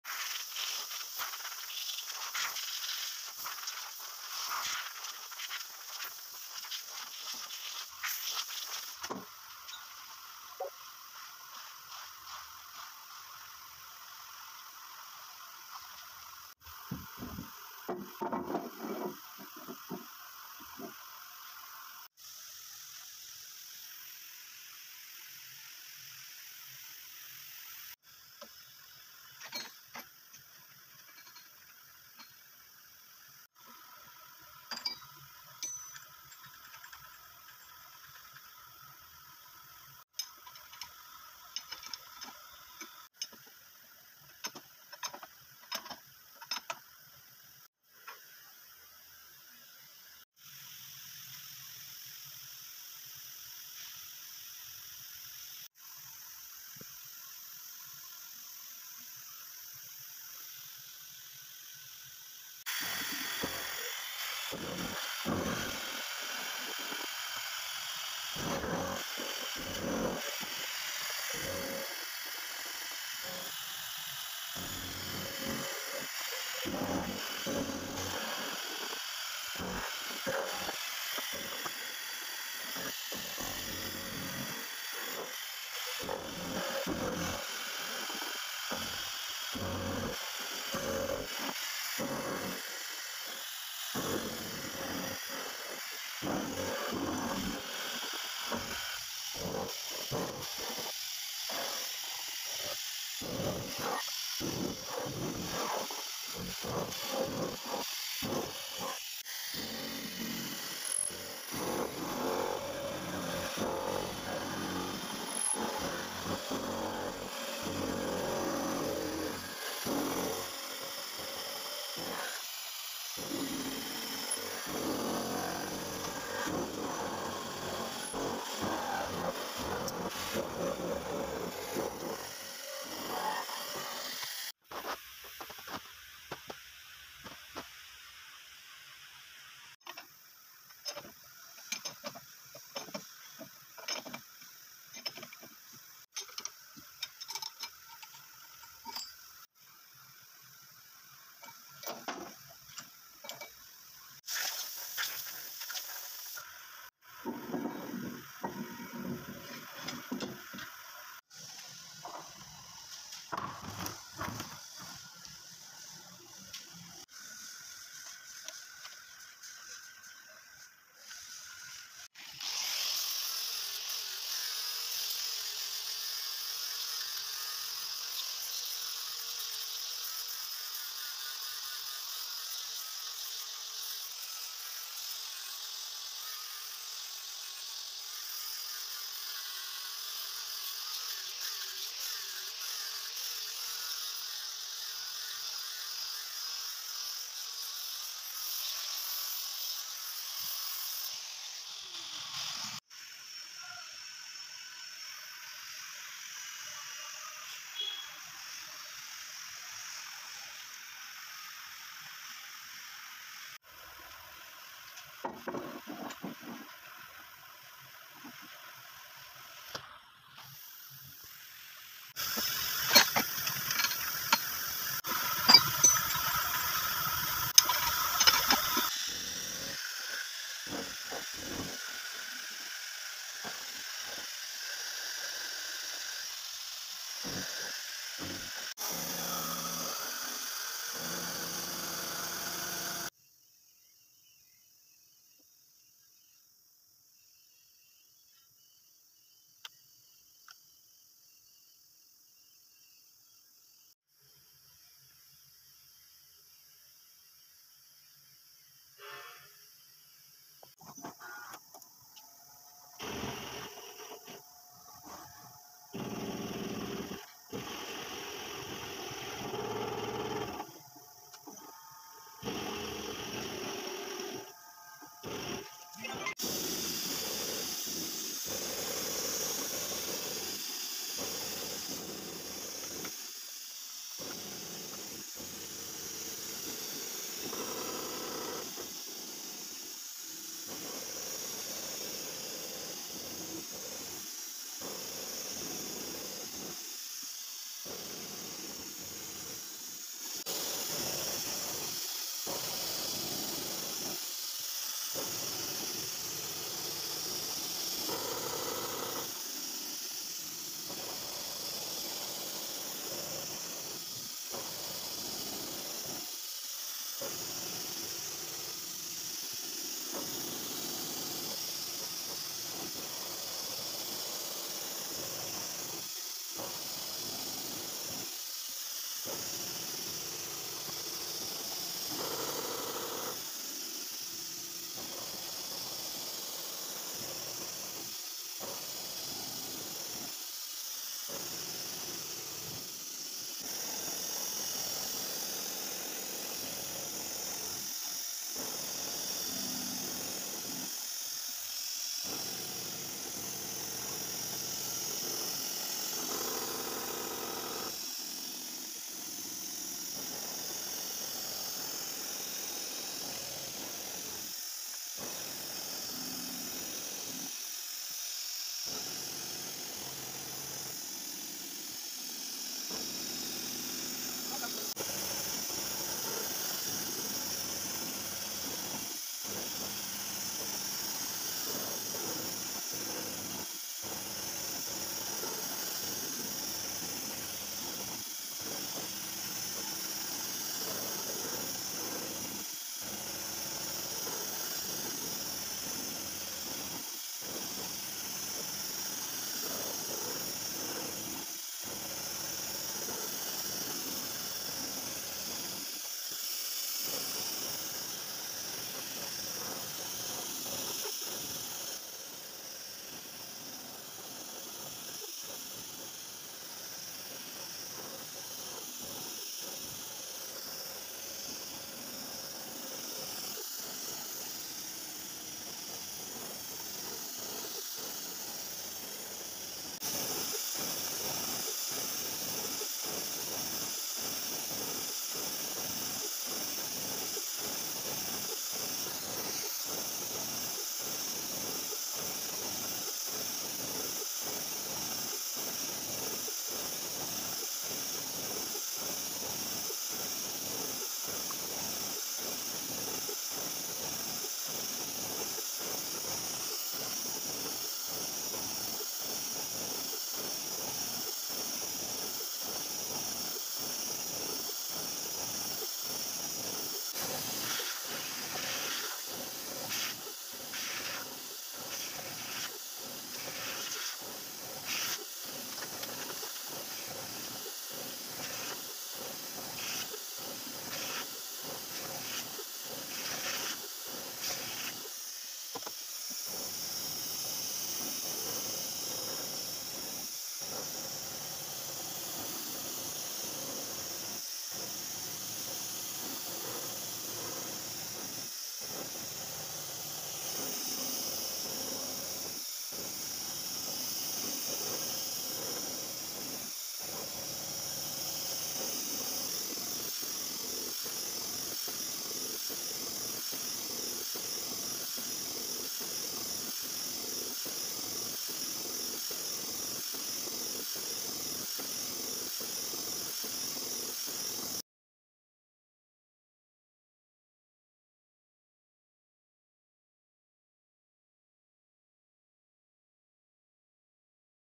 Full Process of Clamping and Machining Part 1
Full Process of Clamping and Machining a Job . Part 1 Clamping Grinding # Programming Designing 3D Animated Operating ToolSelection CNC Milling Vmc Pakistan LG Hartford #1000